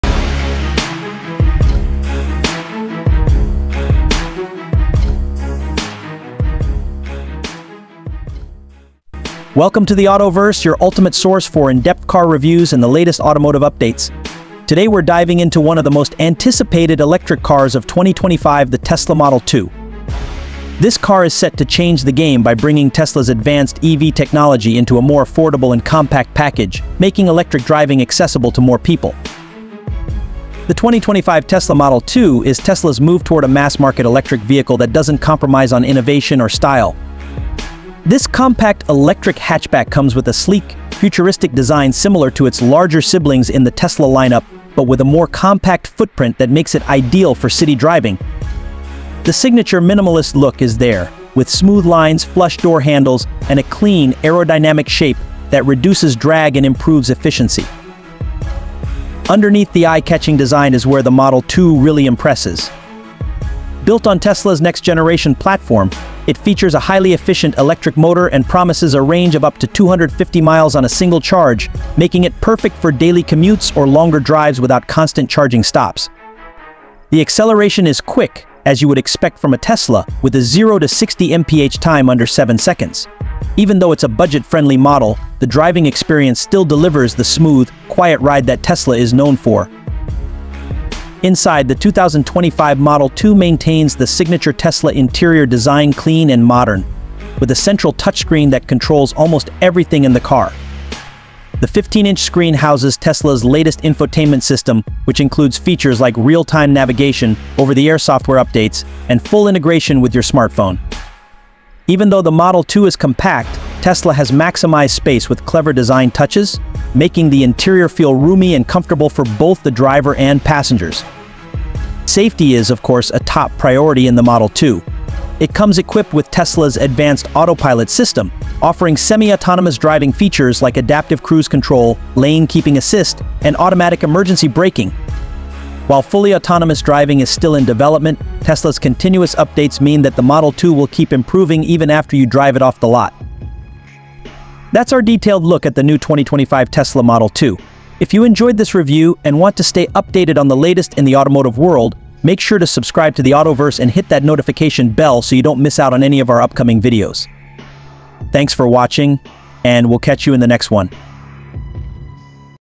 2025 Tesla Model 2: The Affordable Electric Revolution You've Been Waiting For!
2025 Tesla Model 2: The Affordable Electric Revolution You've Been Waiting For!

Get ready for the 2025 Tesla Model 2—Tesla’s groundbreaking affordable electric car that’s set to revolutionize the industry! Discover its jaw-dropping range, impressive features, and why it’s making waves across the globe. Could this be the most accessible Tesla yet?

In this exclusive video, we dive deep into the specs, design, and everything you need to know about Tesla's new electric powerhouse. From game-changing battery technology to sleek new features, the Model 2 is primed to disrupt the EV market.